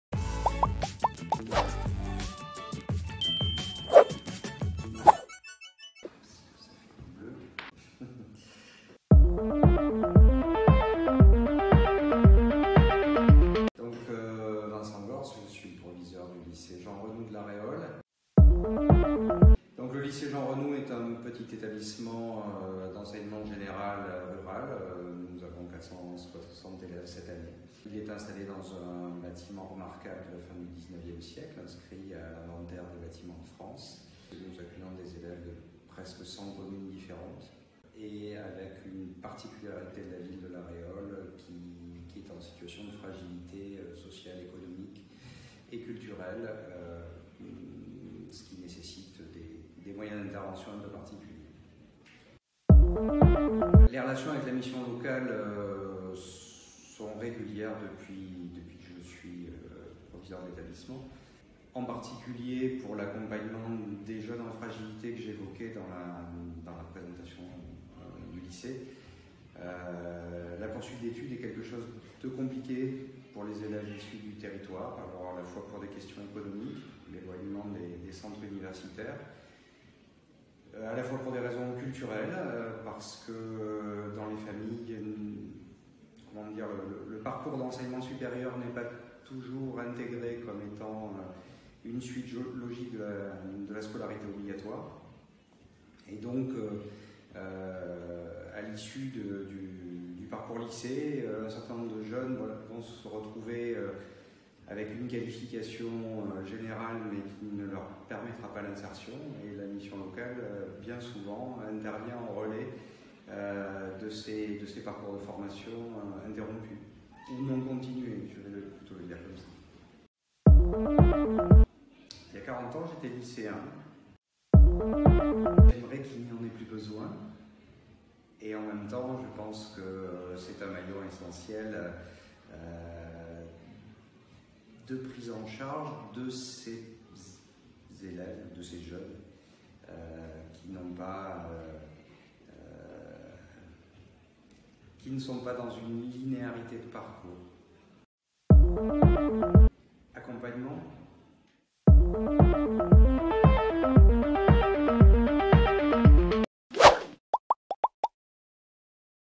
40 ans des Mission Locales -Vincent GORSE Proviseur Lycée Jean RENOU à La Réole
Pour fêter les 40 ans des Missions Locales, nous sommes partis à la rencontre d'acteurs, de partenaires, de jeunes, de conseillers qui ont participé à la création et au développement de la Mission Locale Sud Gironde.